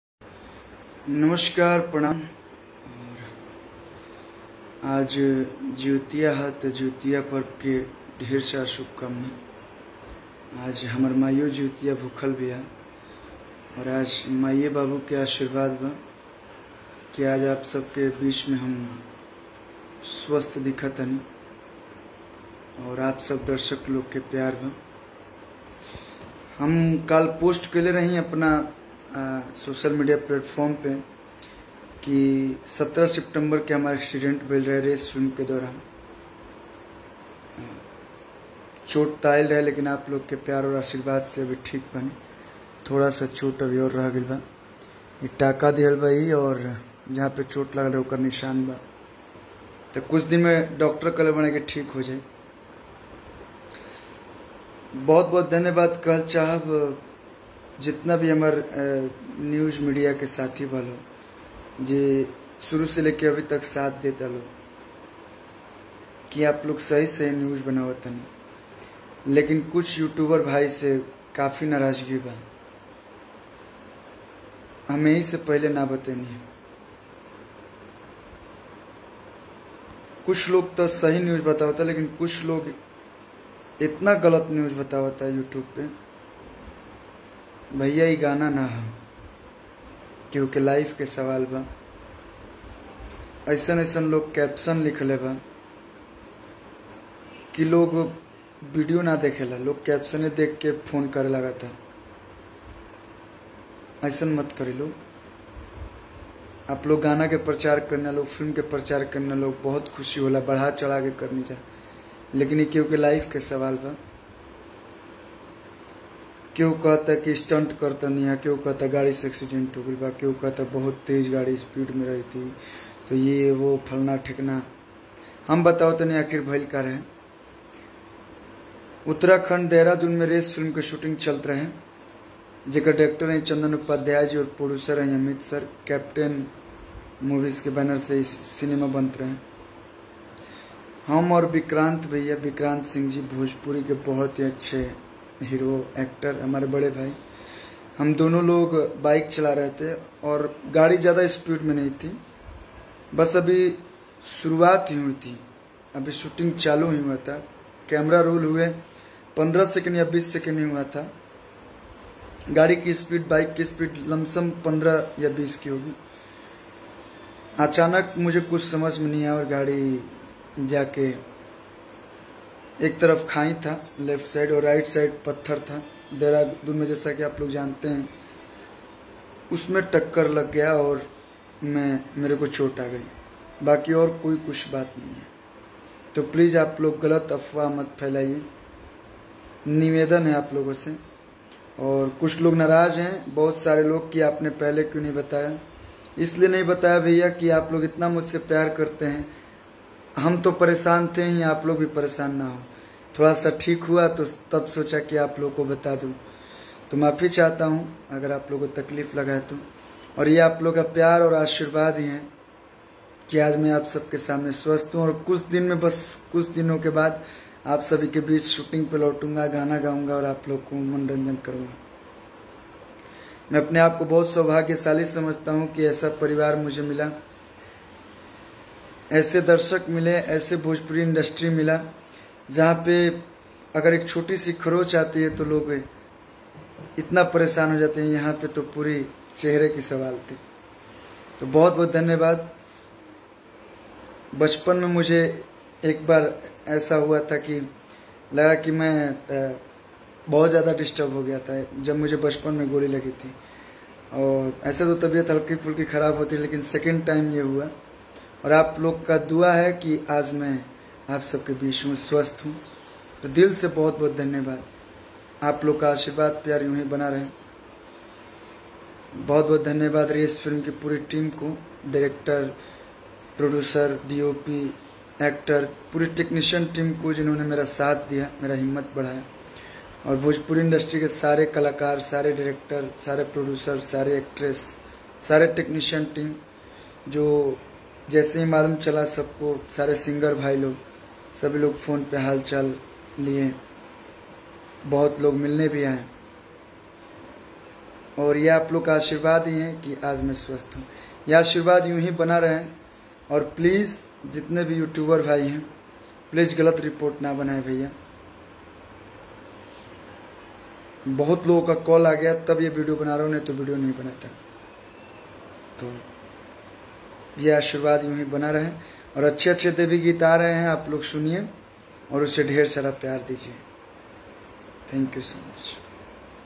Kallu accident के बाद Live आकर अपने दर्शकों को क्या बोले || Kallu car Accident Video 2021
👉 ध्यान दे :- इस वीडियो का मकसद लोगो को ख़बर और जानकारी देना है न की किसी को ठेस पहुँचाना.